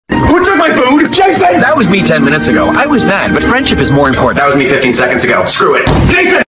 "Clef's Sandwich" an SCP Foundation Gacha Life 2 short
The character in this video was written by DrClef. I got the audio off of CapCut.